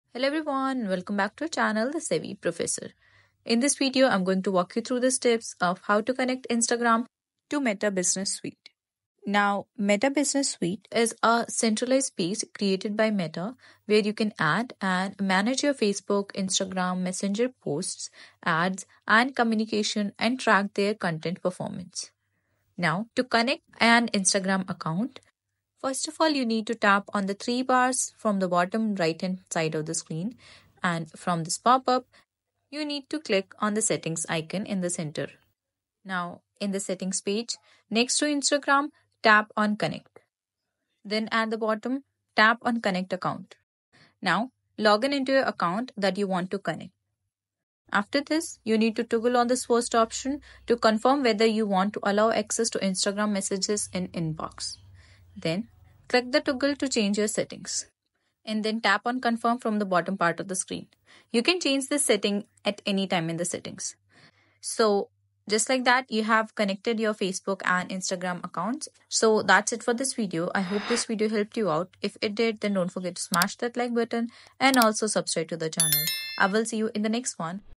How To Connect Instagram To Meta Business Suite (How To Link Instagram To Meta Business Suite)
How To Connect Instagram To Meta Business Suite (How To Link Instagram To Meta Business Suite). In this video tutorial I will show you how to connect Instagram to Meta Business Suite.